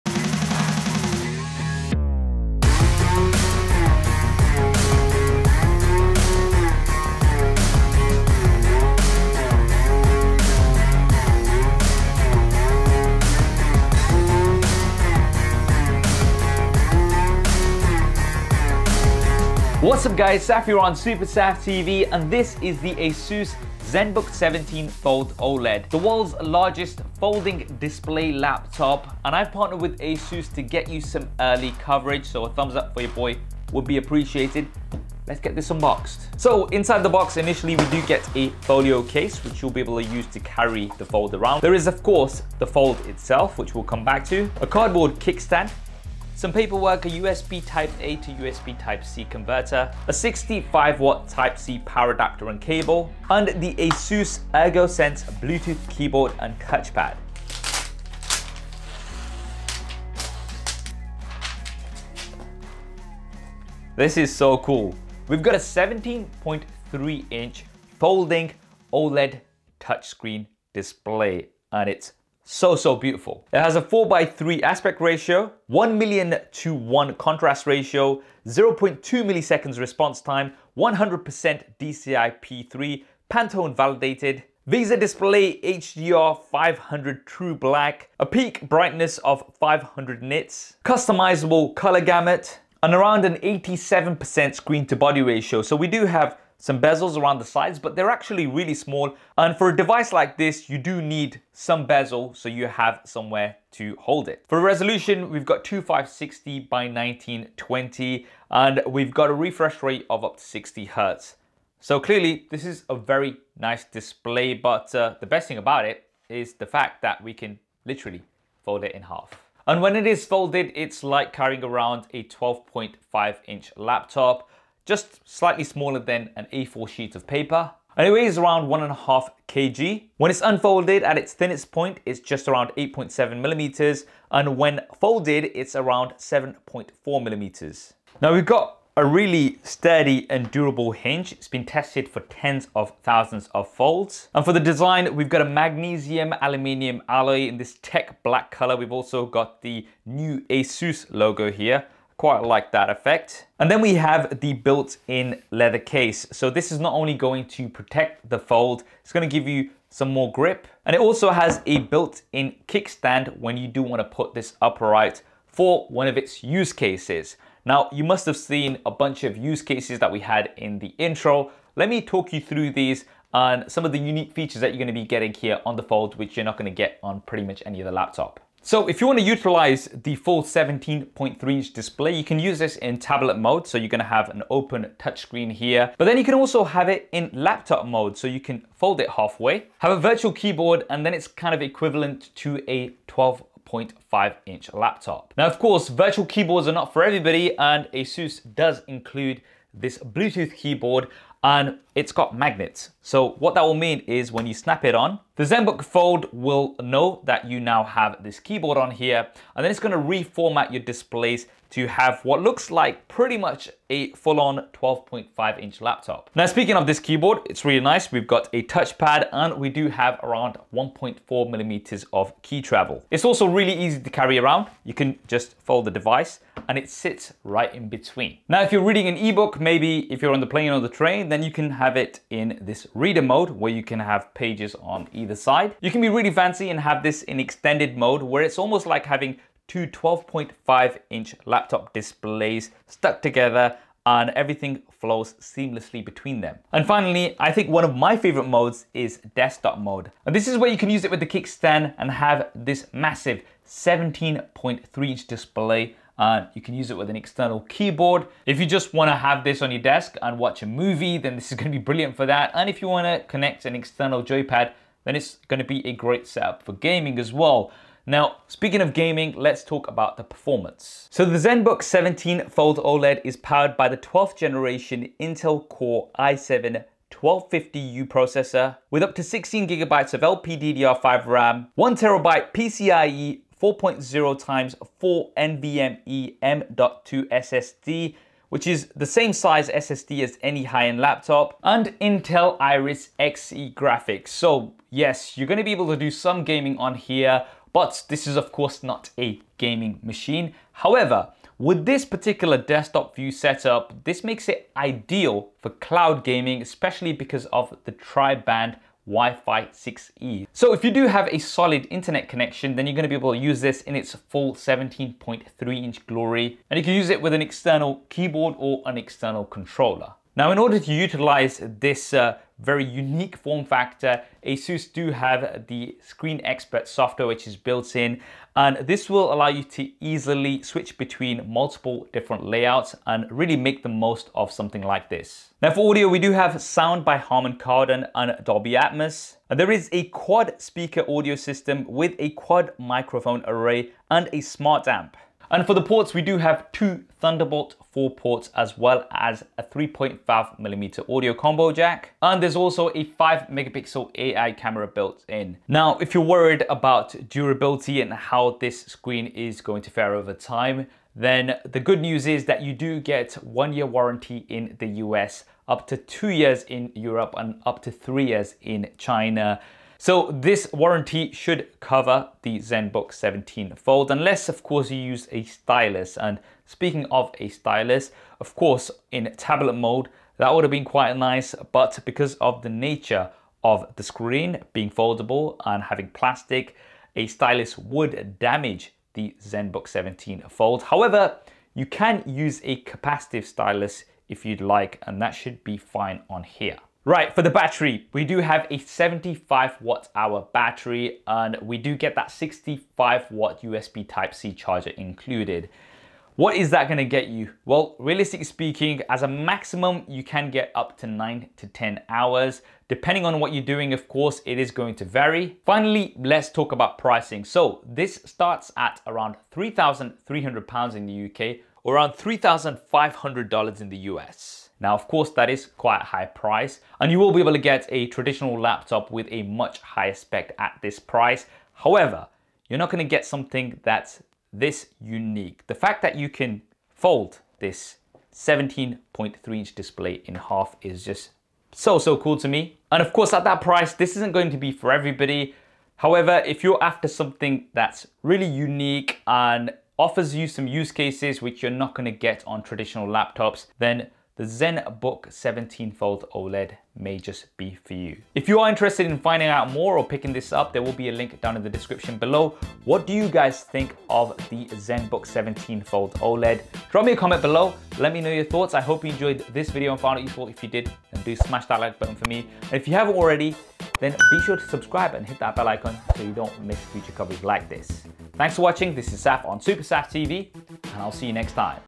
Zenbook 17 Fold - The World's First 17" Foldable Laptop!
ASUS Zenbook 17 Fold OLED (UX9702AA) Unboxing, First Look and Impressions.

The 2022 ASUS Zenbook 17 Fold OLED is the World’s First 17" Foldable Screen Laptop featuring a 17.3” 60Hz OLED Folding Touch display, weighs 1.5kg with a Magnesium aluminum alloy chassis and built-in leather kickstand, powered by the 12th Generation Intel Core™ i7-1250U Processor, Up to 16GB LPDDR5-5200 MHz RAM, 1TB PCIe 4.0 x 4 NVMe M.2 SSD, Intel Iris Xe Graphics, 75Wh Battery with 65W PD Type-C Charging, comes with an ASUS ErgoSense Bluetooth Keyboard and Price is from £3299/$3499.